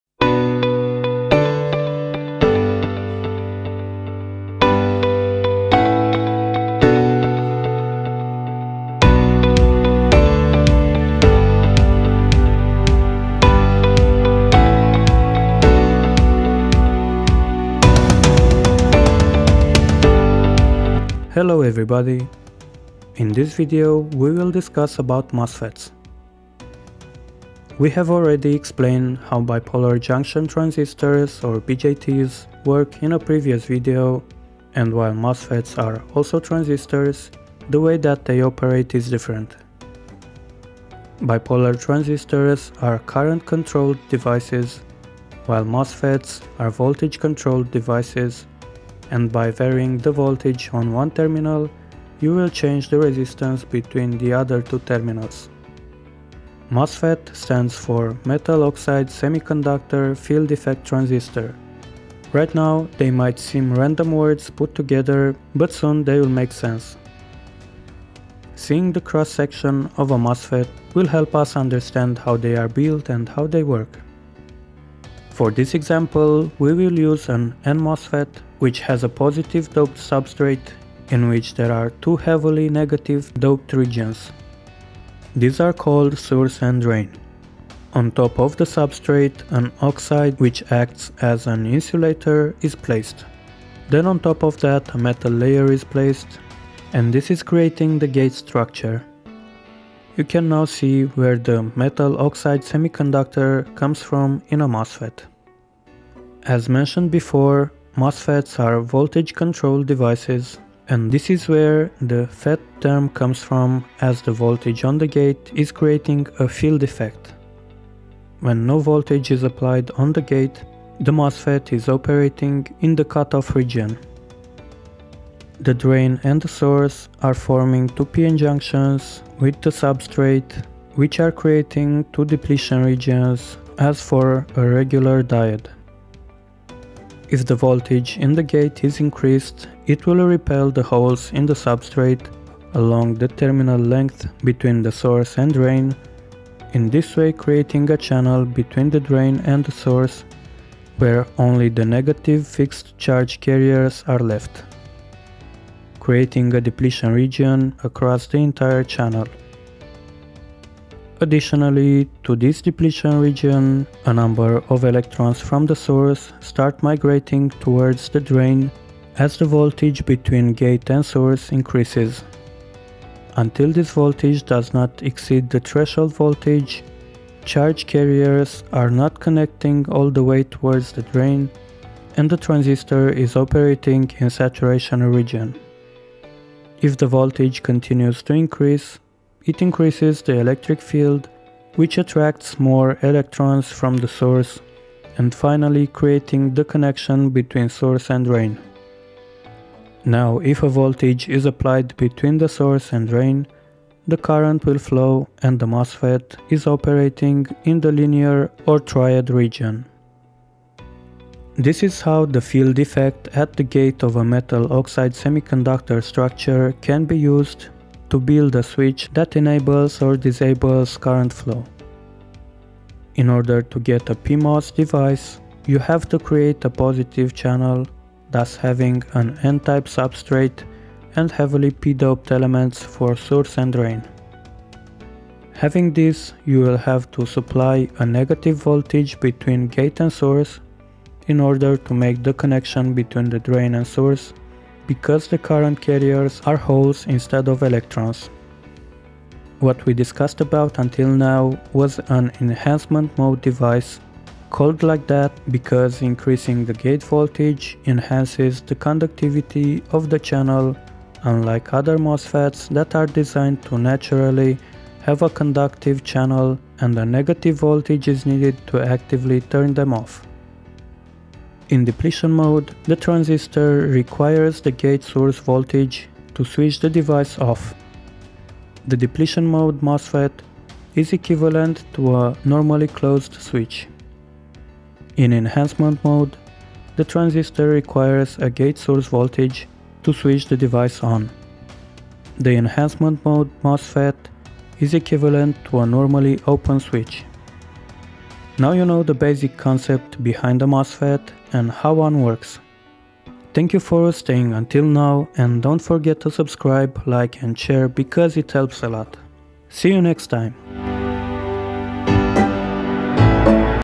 What is a MOSFET and how it works? | Explanation with Animations
In this video I am explaining how MOSFETs work and what is the principle behind a Metal Oxide Semiconductor Field Effect Transistor using it's cross-section and basic electronics knowledge.

Table of Contents:
0:00 Intro
0:28 Difference between BJTs and MOSFETs
0:55 What is a MOSFET
1:08 Physical construction of an N MOSFET
1:57 How the FET works
3:43 P MOSFET construction and working
4:09 Enhancement VS Depletion type MOSFETs
4:58 Outro